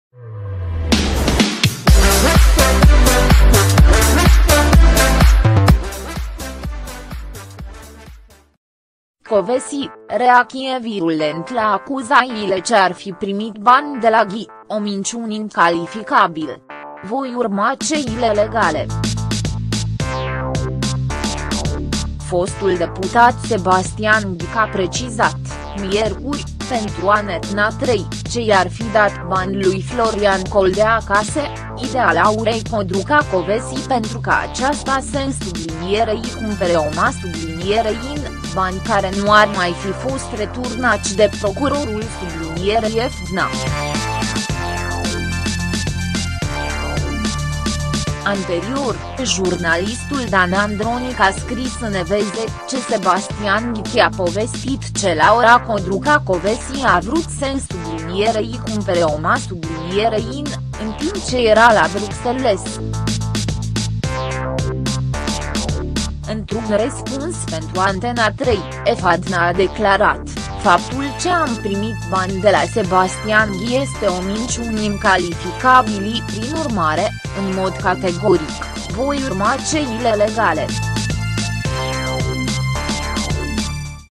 Kovesi, reacție VIRULENTĂ la acuzațiile că ar fi primit bani de la Ghiță: 'O minciună
Kovesi, reacție VIRULENTĂ la acuzațiile că ar fi primit bani de la Ghiță: 'O minciună incalificabilă. Voi urma căile legale'